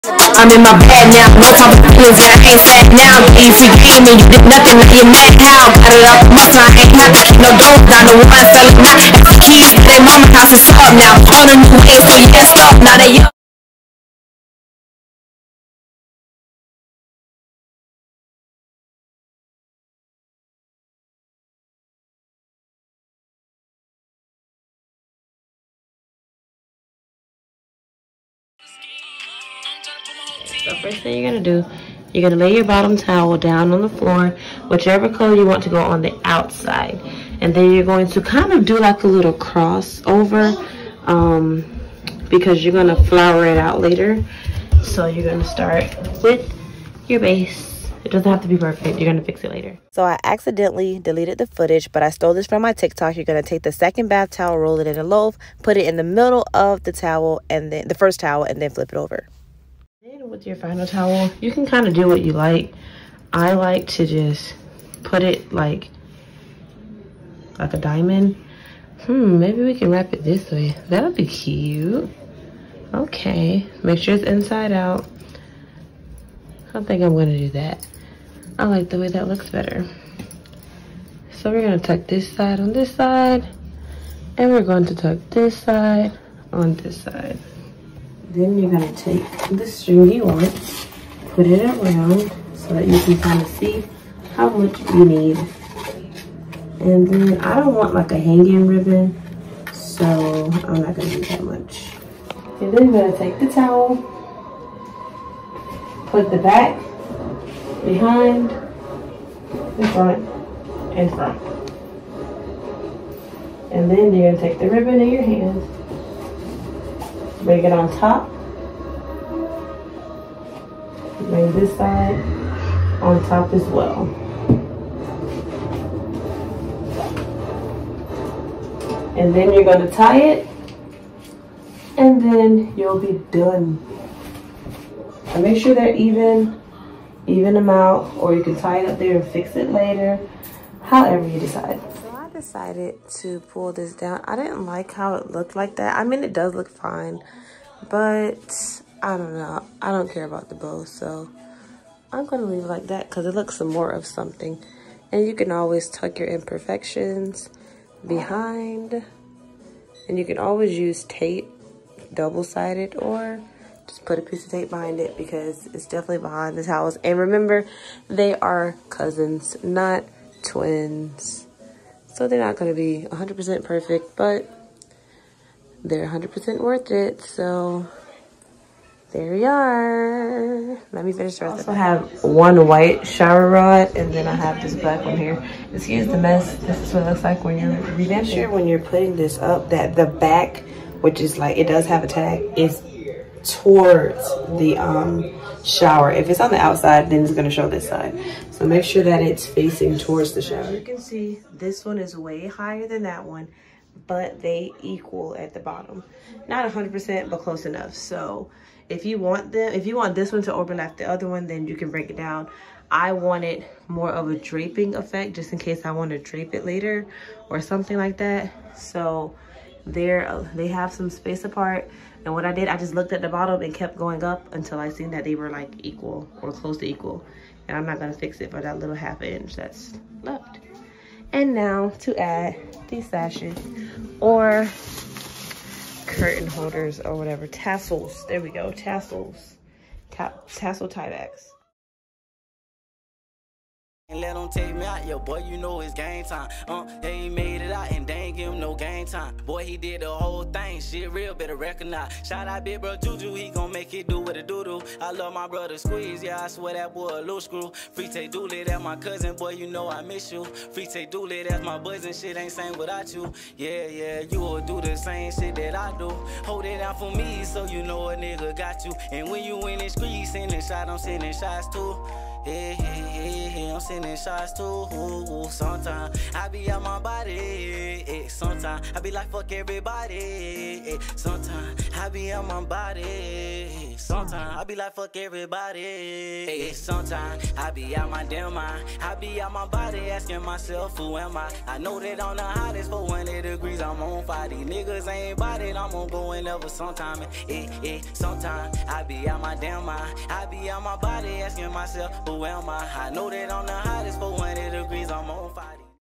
BATHROOM DECORATING IDEAS| How to fold bathroom towels| TOWEL FOLDING IDEAS
Hey guys! It’s been a while! And I’ll be finishing up my home buying series this week!

Everything in this video came from
Walmart
Target
Ross
Burlington

Some items and photos  i already had as you can see in the before and after.

This is for those that just want to touch up their bathrooms and spice it up!